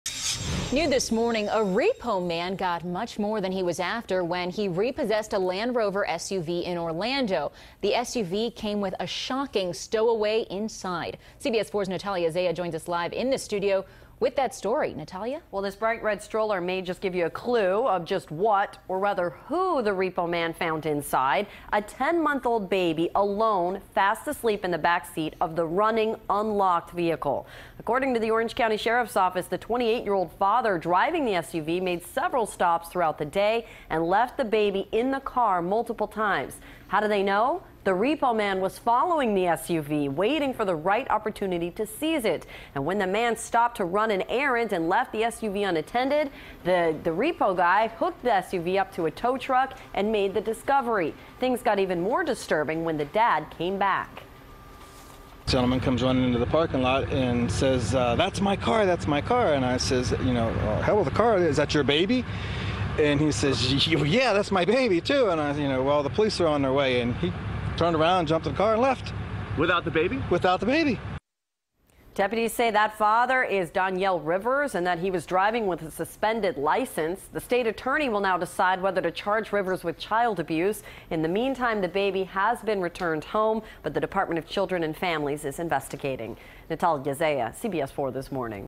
Repo Man Gets More Than He Bargained For
Found baby in Land Rover